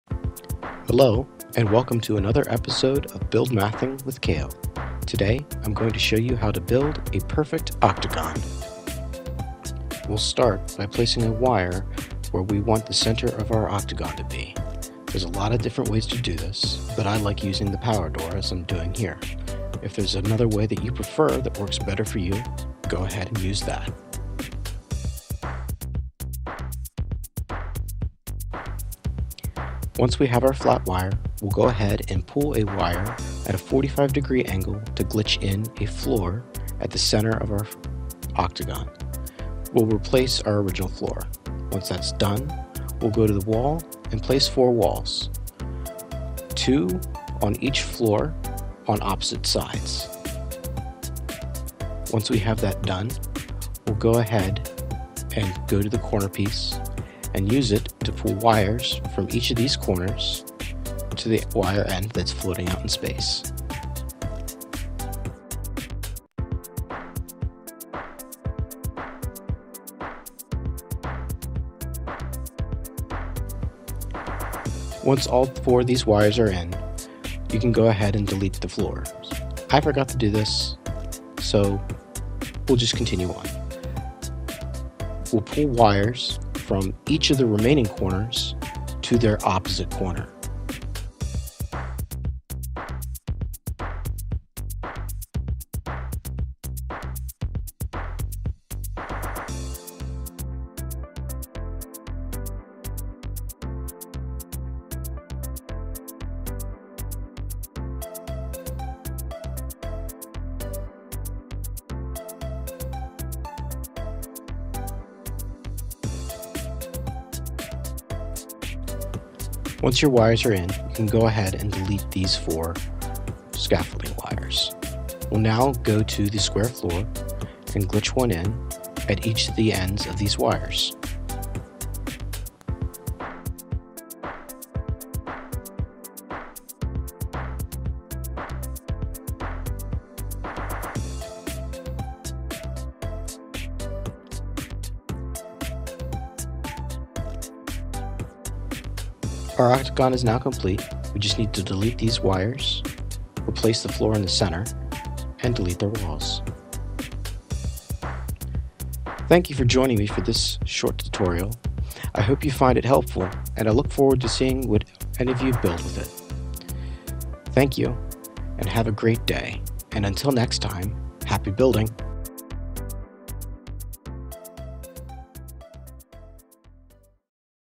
Build Mathing the Perfect Octagon
This tutorial will show you how to build a perfect octagon starting from the center.  it will not cover any of the glitch techniques used.  For tutorials check out Beeblebum's "COMPLETE Glitch Building Guide"